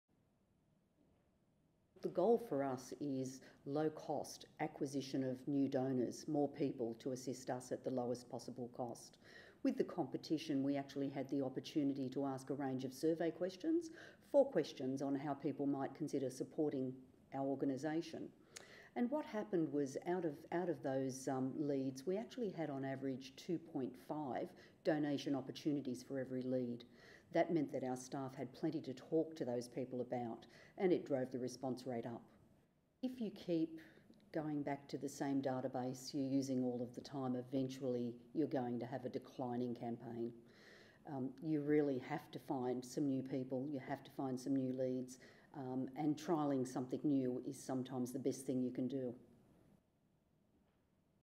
Higher Closing Leads - Golden Leads Formula
In just 11 weeks, Epilepsy Action Australia generated 2030 Donation Opportunities from 822 Donor Leads that Closed at 34.15%!

Epilepsy Action Australia CEO Carol Ireland discusses the benefits of using the Golden Leads Formula through Your Lead Engine to generate High Closing Donor Leads.